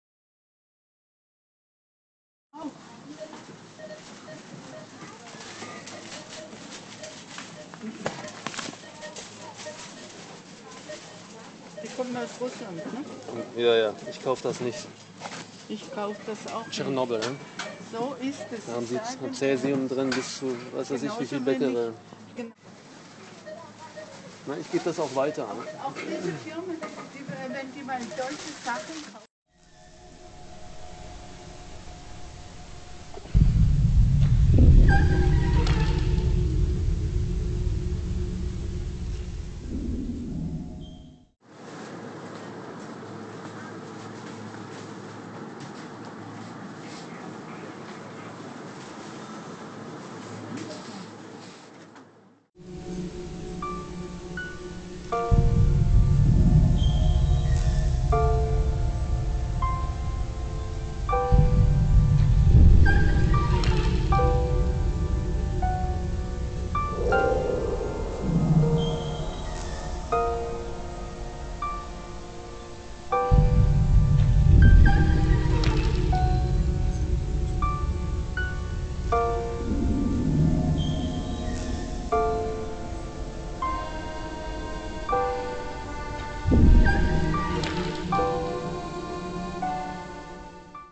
Nuclear Waste in Supermarkets: ALDI 2013
"Calculations based on models used by the International Commission on Radiological Protection (ICRP) show that dietary intake of the maximum amount of radioactive contamination permitted in the EU and Japan would lead to at least roughly 150,000 fatalities in Germany each year. Other calculation models reach vastly higher figures. If the entire German population were to eat foods exposing individuals to only 5 percent of the contamination currently allowed in food imports from Japan, at least 7,700 fatalities could be expected; this figure doesn't even include the secondary consequences of a wide range of greatly varying diseases and genetic disorders."